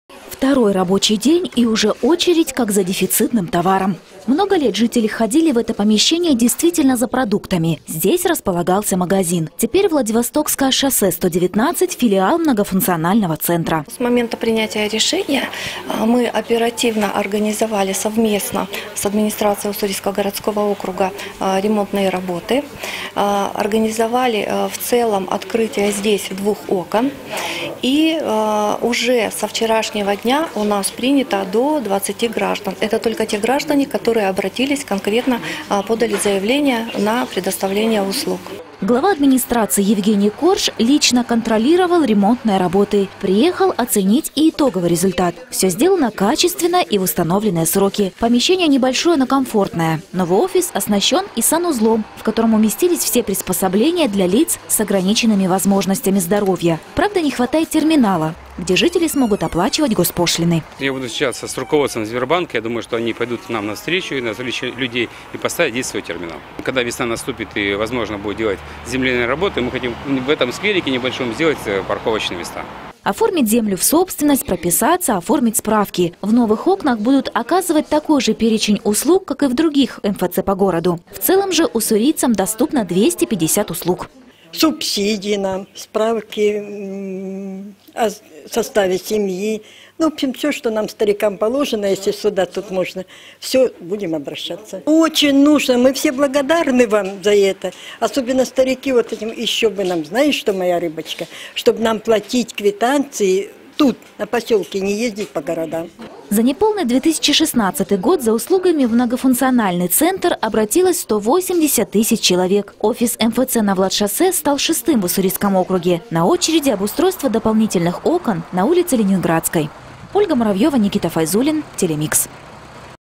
Новый МФЦ открылся на Сахпоселке
Офис Многофункционального центра открылся на Владивостокском шоссе. Этот филиал стал шестым в округе. И теперь жителям микрорайона за государственными и муниципальными услугами не нужно ехать в центр.